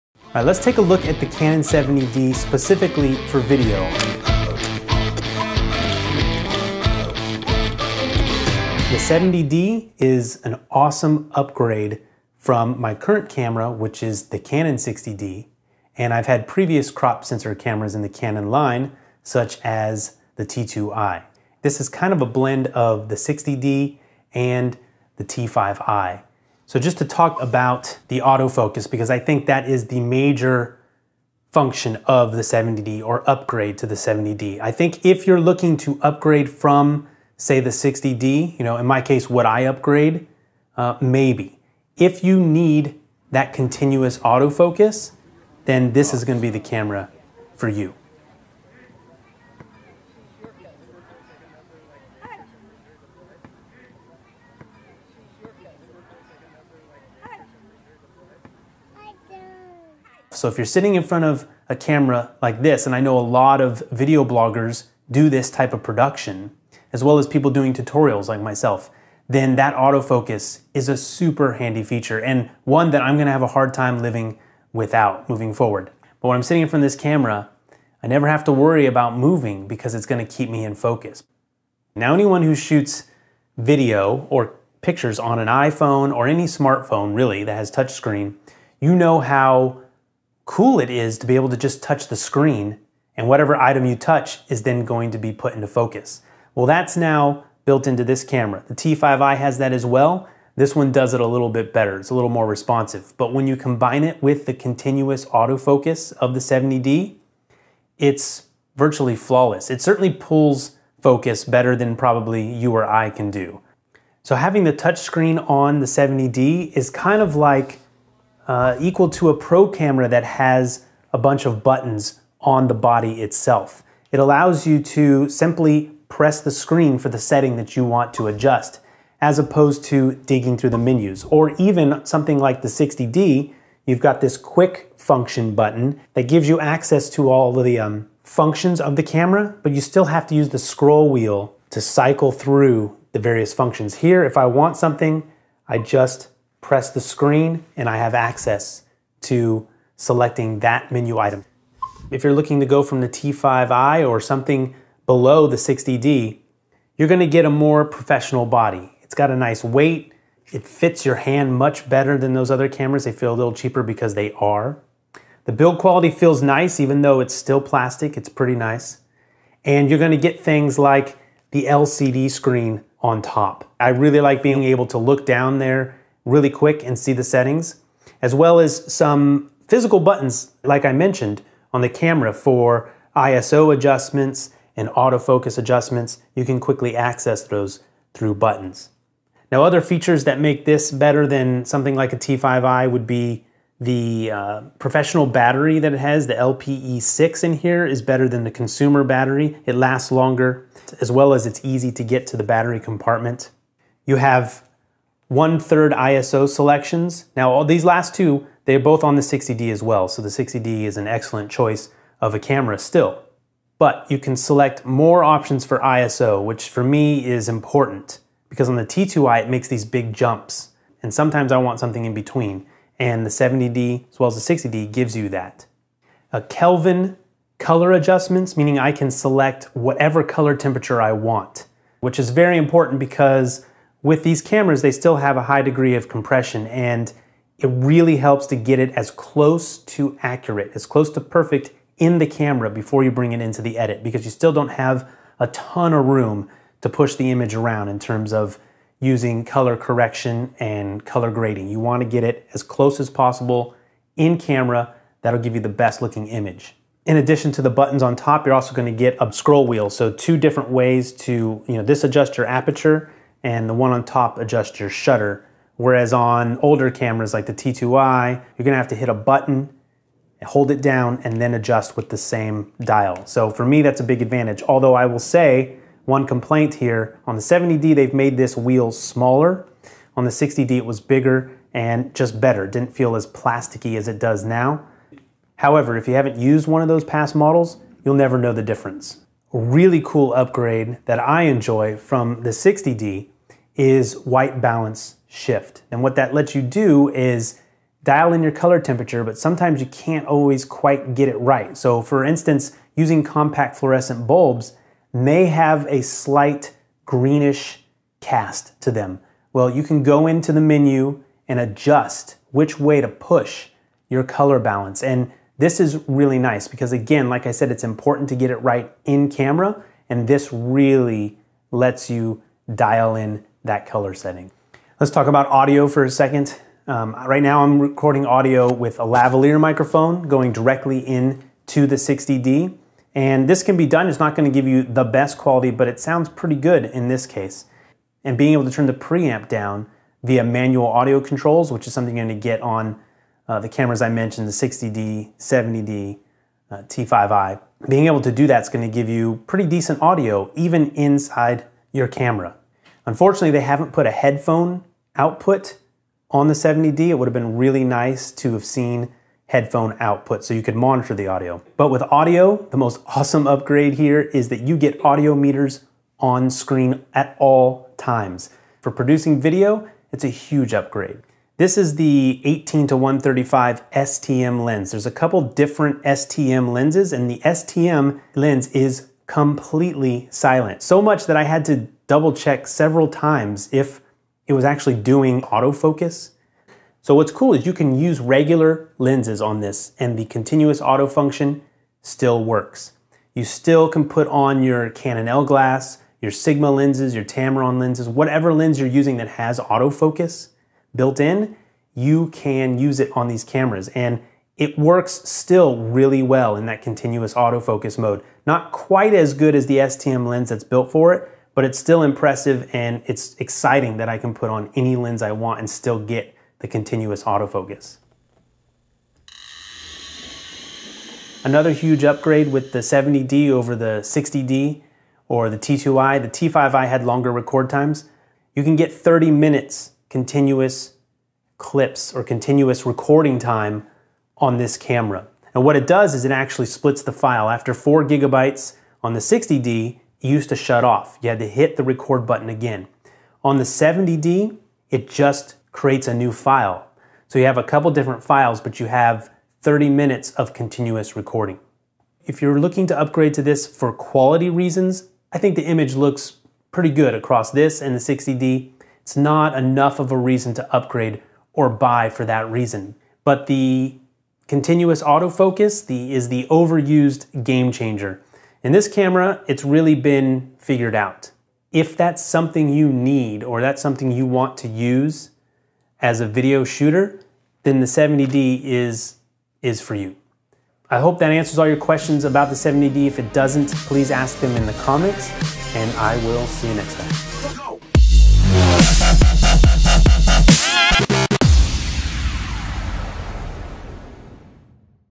Canon 70D Video DSLR
This is canon 70D camera. its feature is just awesome.
When someone is going to capture a photo, it looks like a real photo.
It is really a good camera. you can collect it.